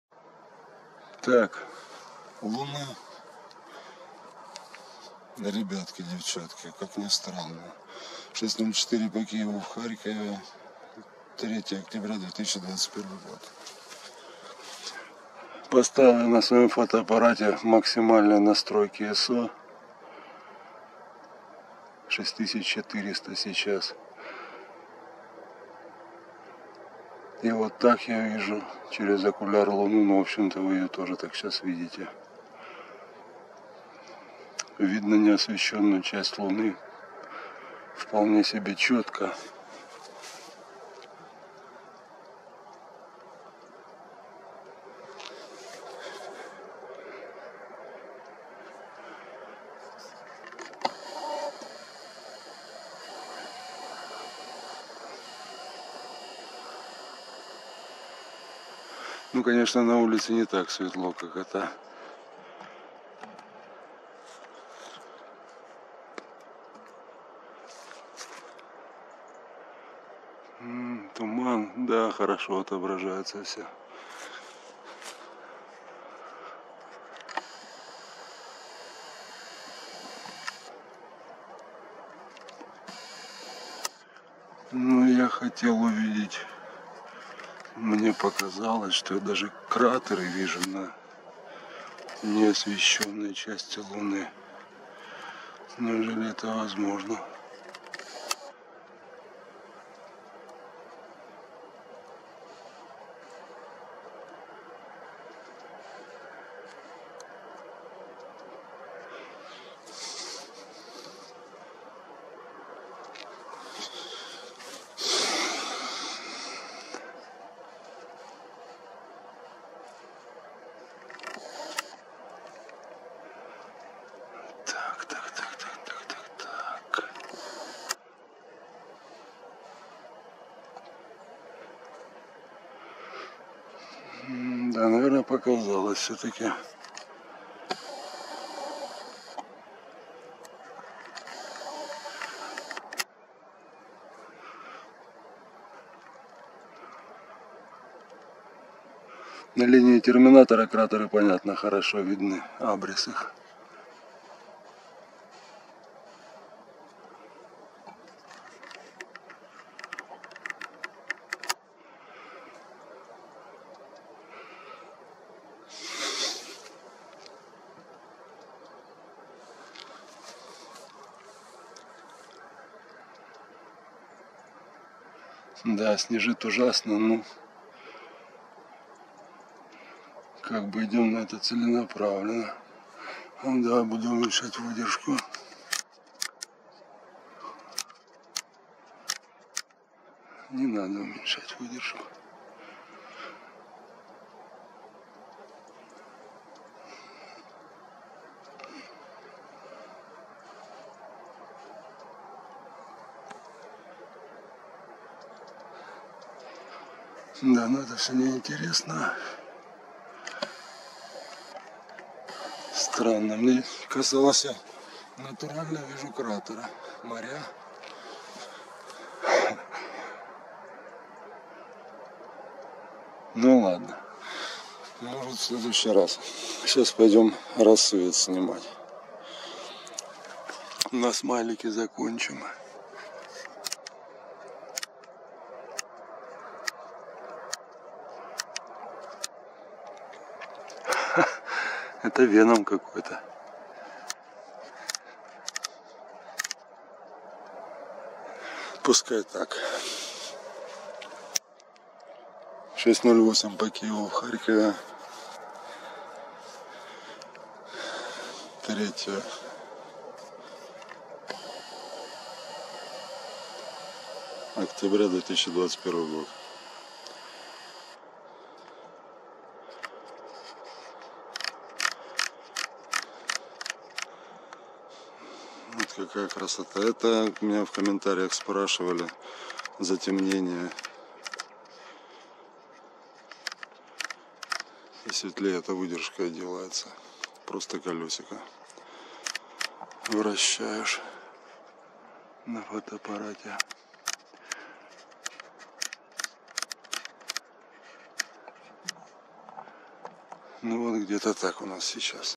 Кратеры на не освещённой части Луны 3 октября 2021
Серповидная Луна  на рассвете 3 октября 2021 - видно абрисы кратеров на не освещённой части Луны.